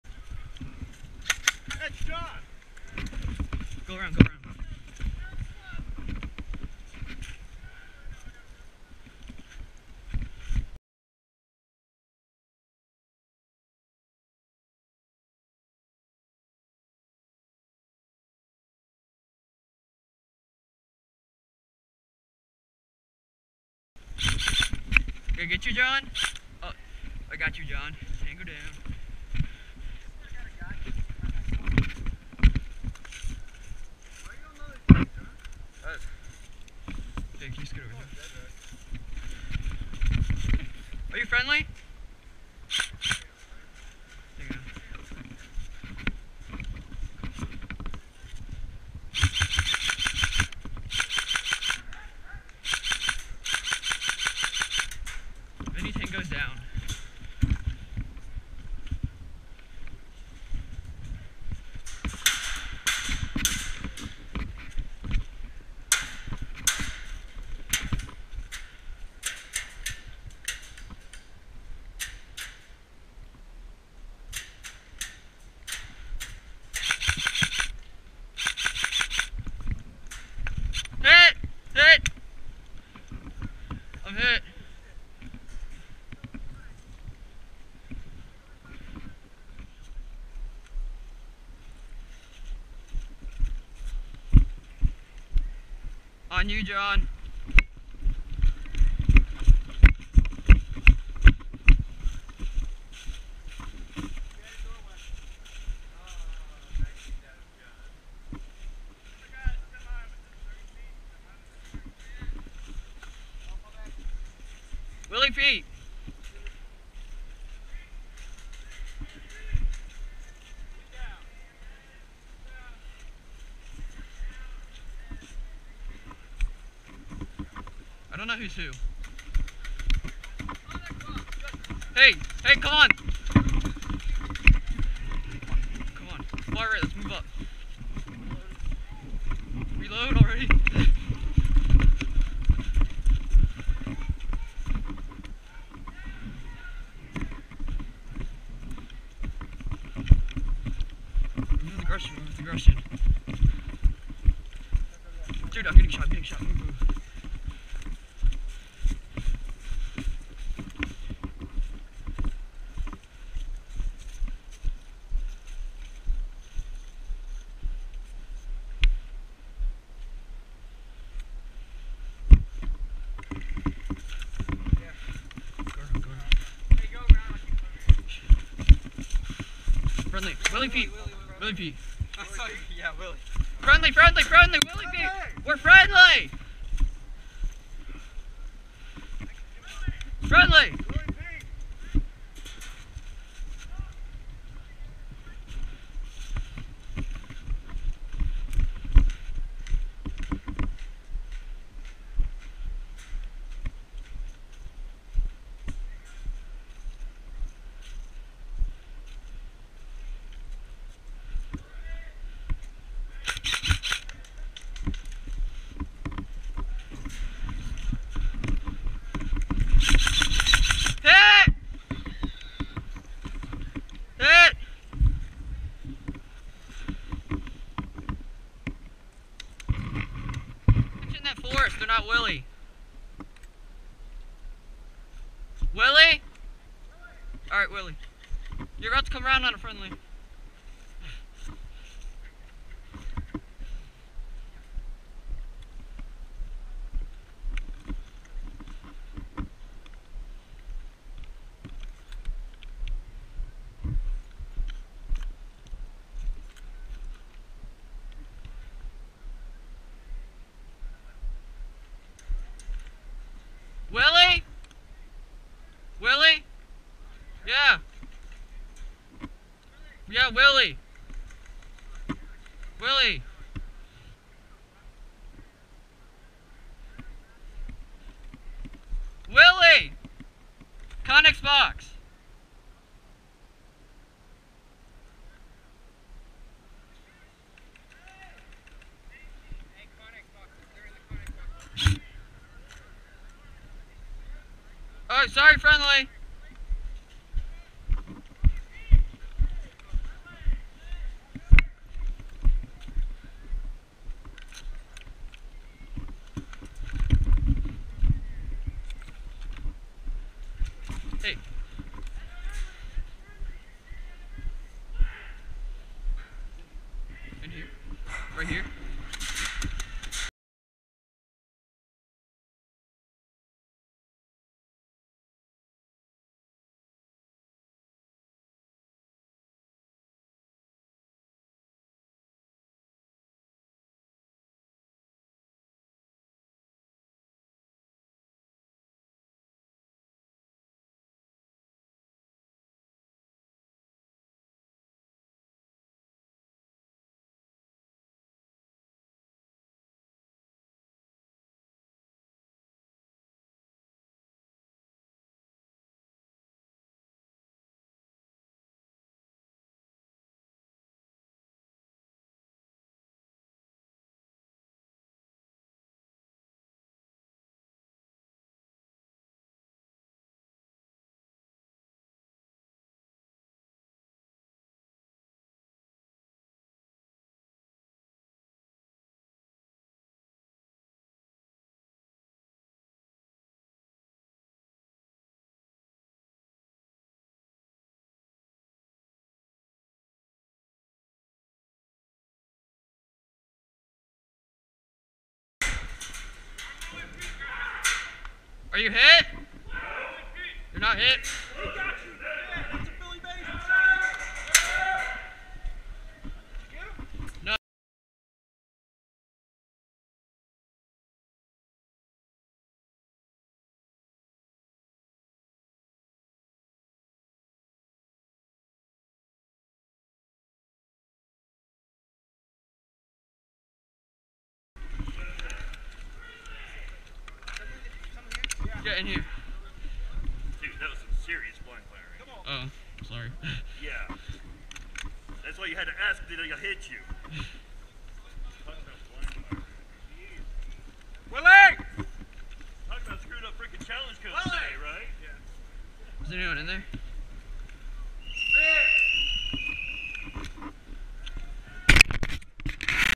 Ballahack Airsoft Town Chaos 11/8/15 GoPro Helmet Mount
Primary: G and G Combat Machine (Rental)
Secondary: Issc M22 CO2 GBB

I hope you enjoy watching it as much as I like posting it. Please consider liking, commenting, and sharing. More stuff below